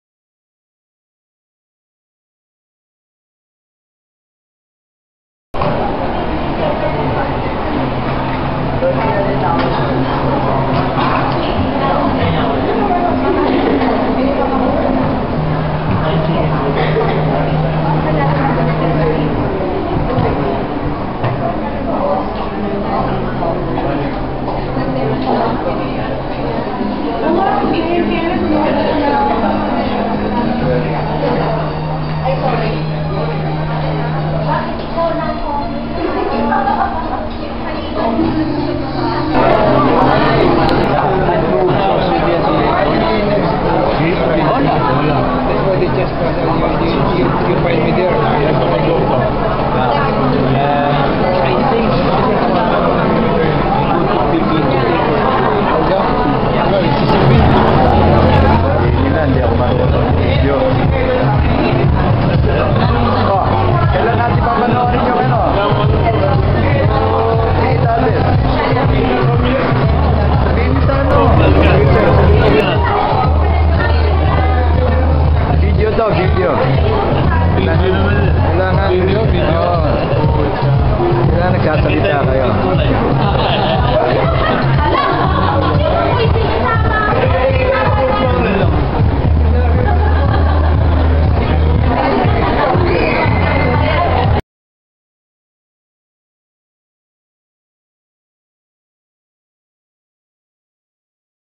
Raul Isidro
one man exhbition of recent paintings at Galeria Nicolas, Greenbelt 4., Makati